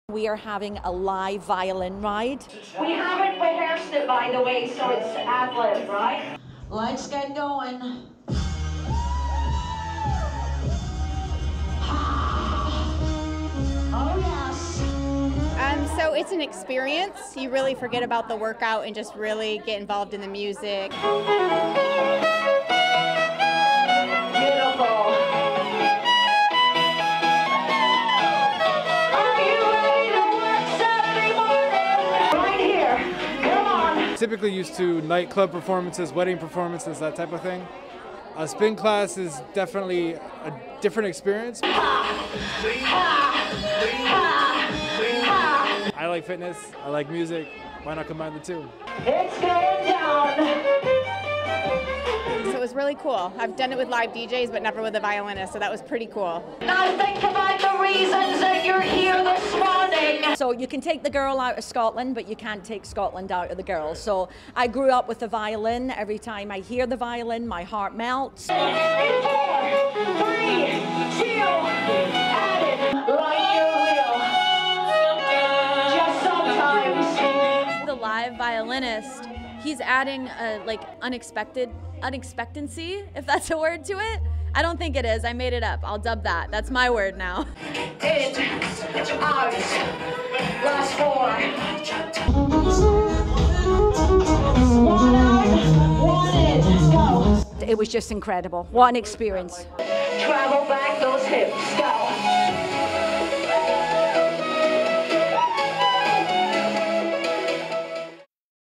Violinist plays spin class in Wellington
CycleBars are franchised spin studios. They are known for their dance club vibe with a dark studio, flashing lights, and loud music. WPTV NewsChannel 5's Entertainment Reporter T.A. Walker shows us how they added a live violinist into the mix this morning in Wellington.